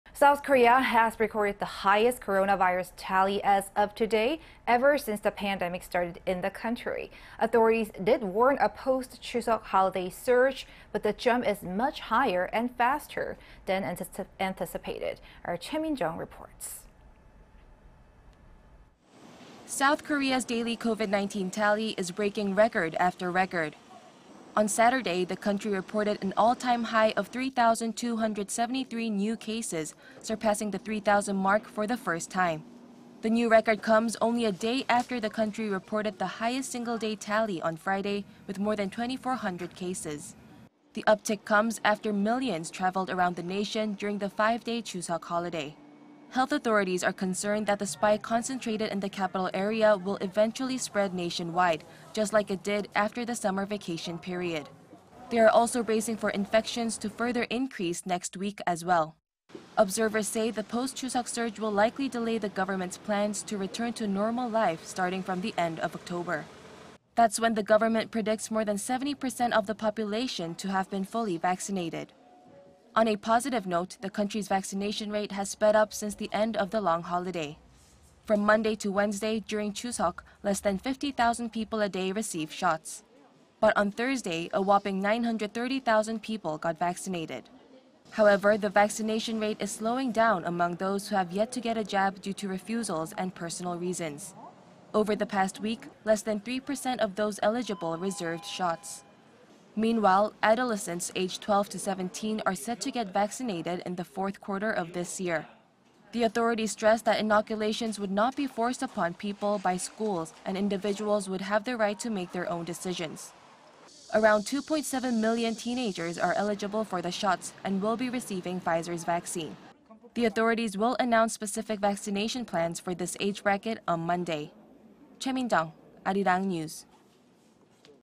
S. Korea reports record-high 3,273 new cases of COVID-19 on Saturday
신규확진 3천273명, 연일 폭증에 첫 3천명대… 위드 코로나 조기전환 힘들듯... 다시 속도내는 백신 접종…미접종자 예약은 1주일 지났지만 2% 불과... 초6∼고2 백신 접종, 강요 아닌 자율 접종 방침

South Korea has recorded the highest coronavirus tally today ever since the pandemic started in the country.
Authorities did warn a post-Chuseok holiday surge, but the jump is much higher and faster than anticipated.
Choi Min-jung reports.

South Korea's daily COVID-19 tally is breaking record after record.
On Saturday, the country reported an all-time high of 3-thousand 2 hundred 73 new cases, surpassing the 3-thousand mark for the first time.
The new record comes only a day after the country reported the highest single-day tally on Friday with more than 24-hundred cases.
The uptick comes after millions traveled around the nation during the five-day Chuseok holiday.

Health authorities are concerned that the spike concentrated in the capital area, will eventually spread nationwide, just like it did after the summer vacation period.
They are also bracing for infections to further increase next week as well.
Observers say the post-Chuseok surge will likely delay the government's plans to return to normal life starting from the end of October.
That is when the government predicts more than 70-percent of the population to have been fully vaccinated.

On a positive note, the country's vaccination rate has sped up since the end of the long holiday.
From Monday to Wednesday, during Chuseok, less than 50-thousand people a day received shots.
But on Thursday, a whopping 930-thousand people got vaccinated.
However, the vaccination rate is slowing down among those who have yet to get a jab due to refusals and personal reasons.
Over the past week, less than 3-percent of those eligible reserved shots.

Meanwhile, adolescents aged 12 to 17 are set to get vaccinated in the fourth quarter of this year.
The authorities stressed that inoculations would not be forced upon people by schools, and individuals would have the right to make their own decisions.
Around 2-point-7 million teenagers are eligible for the shots and will be receiving Pfizer's vaccine.
The authorities will announce specific vaccination plans for this age bracket on Monday.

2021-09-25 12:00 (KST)